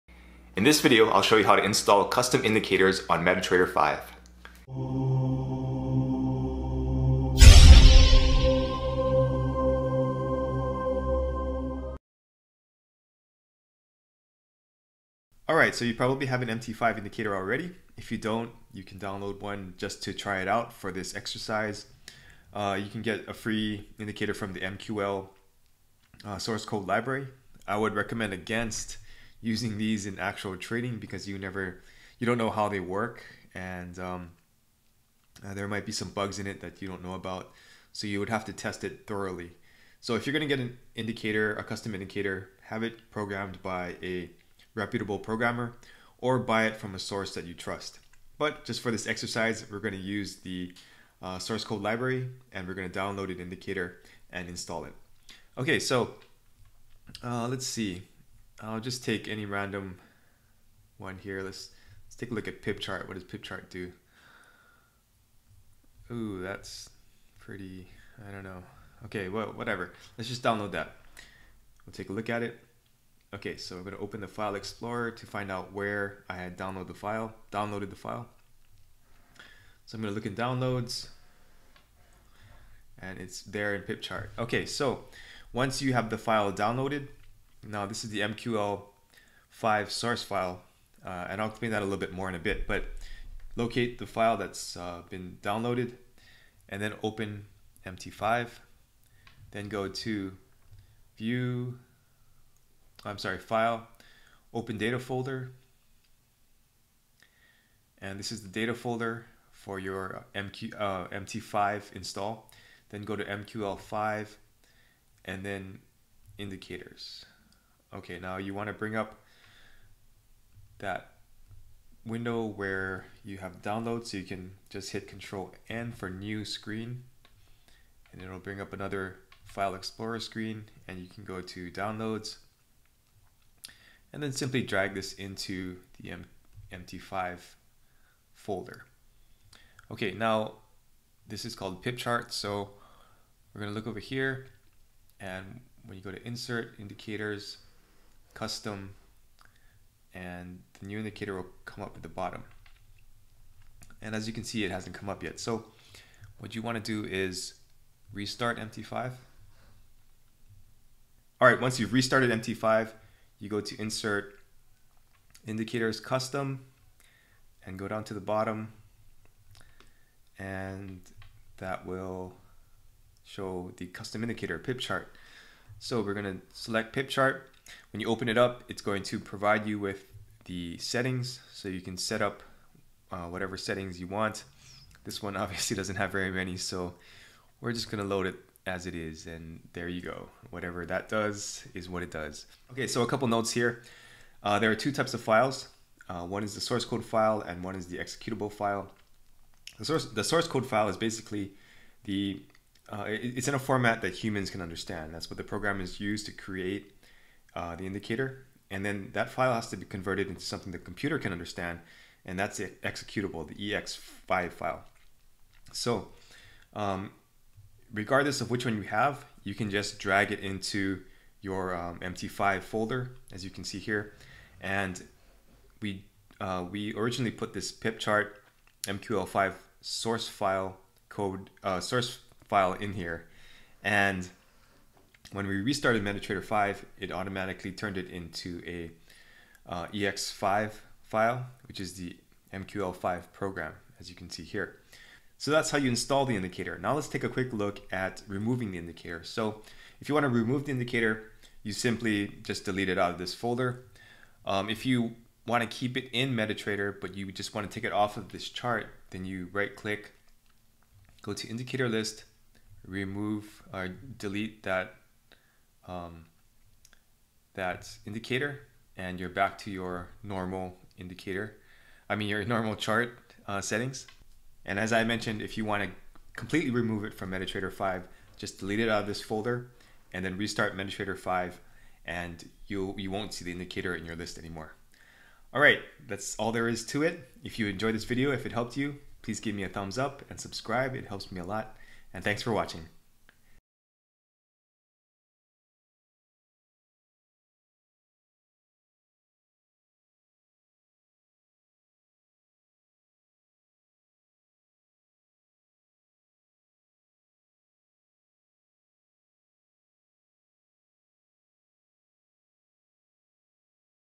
How to Install Indicators on MT5 PC - MetaTrader 5 Tutorial
This simple tutorial teaches you how to install indicators on the MetaTrader 5 trading/charting platform. Also learn how to remove indicators, change the settings and more. Note: Custom indicators can only be installed on the desktop (PC) version of MT5. They cannot be installed on the mobile app.

Nostr: npub1v5ty092cnhtt6aehn0csu9mhfk0yprqz8335ae0y2y07fk95vmmqrn26pk